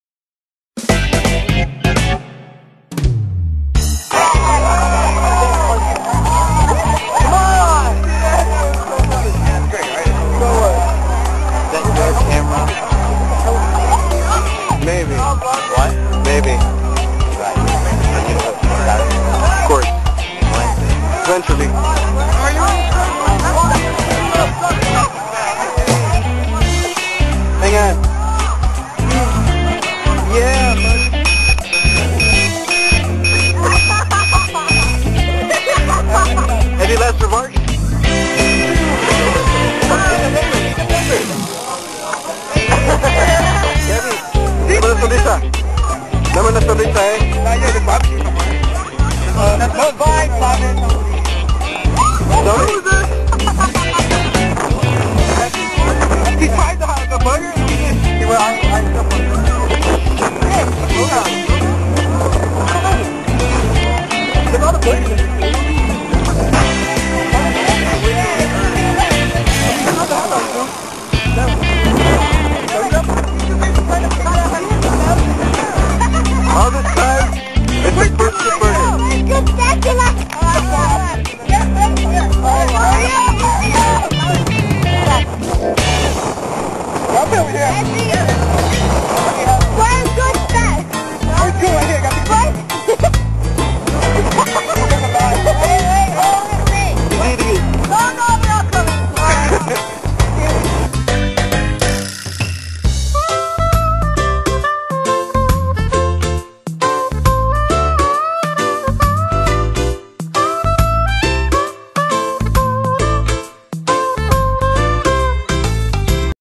AHS EarthDay 2005
Amundsen High Schools yearly Earth Day Festival!
Collage of random clips taken through out the day.
Yeah. Comment if you will, but theres no point to that...but thats up to you..